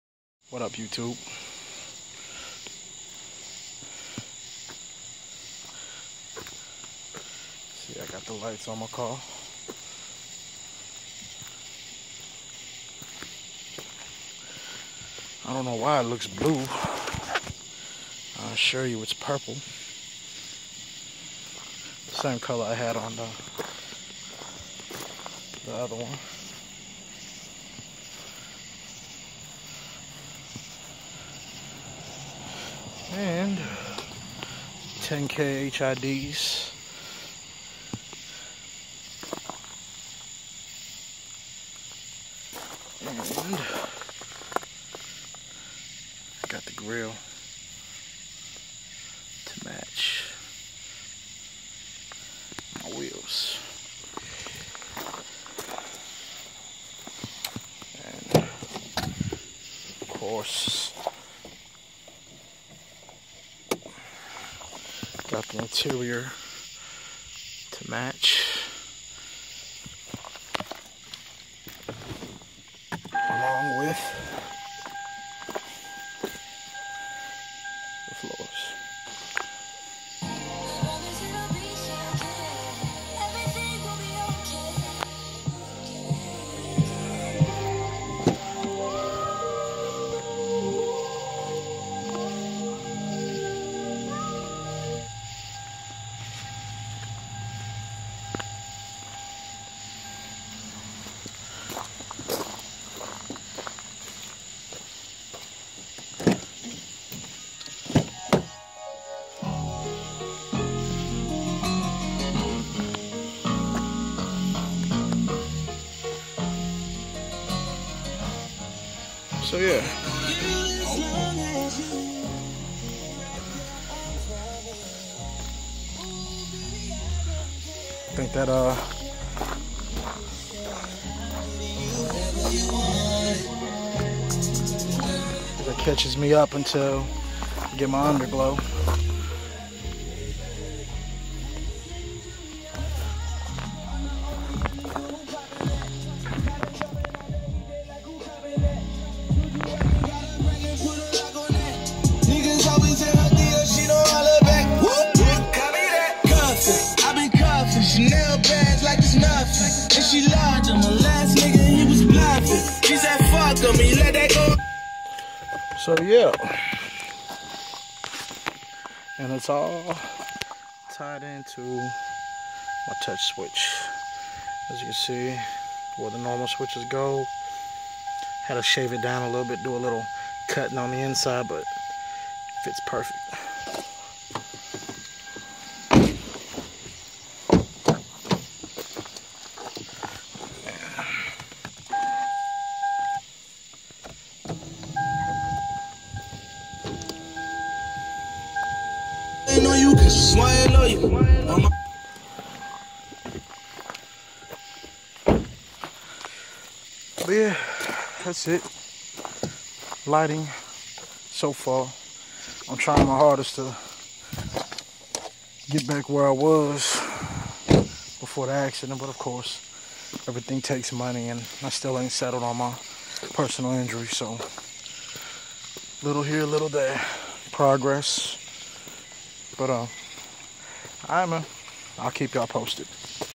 Custom Avalon on 22s (Coco) custom exterior lighting
A little work here and there. Eventually I'll be caught up to where my last Avalon (Ava) left off.